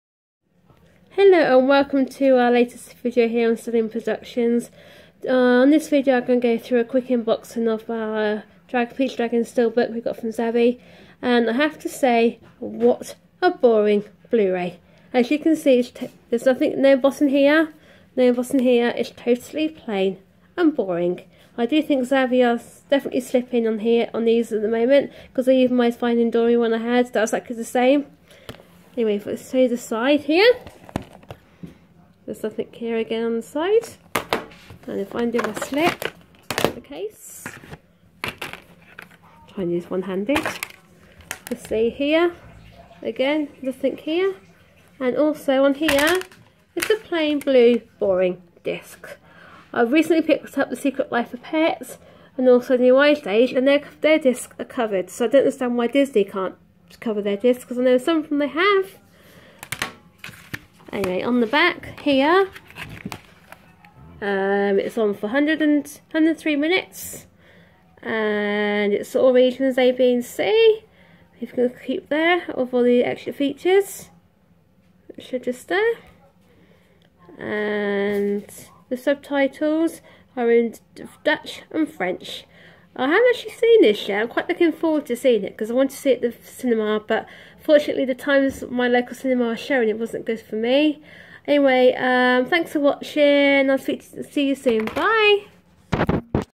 Steelbook Time With Cheryl - Episode 1
Cheryl brings to you her thoughts on the lastest Disney Steelbook Pete's Dragon